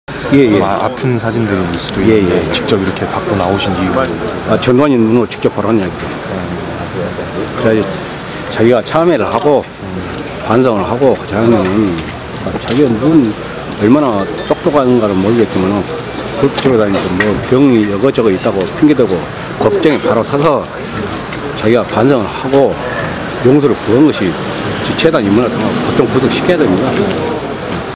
5.18 사진 전시한 구속자회 “전두환 두 눈으로 직접 봐라”
전두환 전 대통령이 5.18민주화운동과 관련해 사자명예훼손 혐의로 재판에 출석할 예정인 11일 오전 광주지방법원 앞에서 5.18민중항쟁 구속자회 회원들이 5.18 당시 참혹한 현장의 모습이 담긴 사진을 전시했다.

5.18민중항쟁구속자회의 김광호 동구지회장은 "전두환이 눈으로 직접 보라 이거다"라며 사진을 "오월영령들을 농락하고 있는 사람을 절대 용서할 수 없다"라고 말했다.
그는 "골프치러 다니면서 병 있다고 핑계나 대는 그 사람이 얼마나 똑똑한지 모르지만 법정에 바로 서서 반성하고 용서를 구하라"라고 강조했다.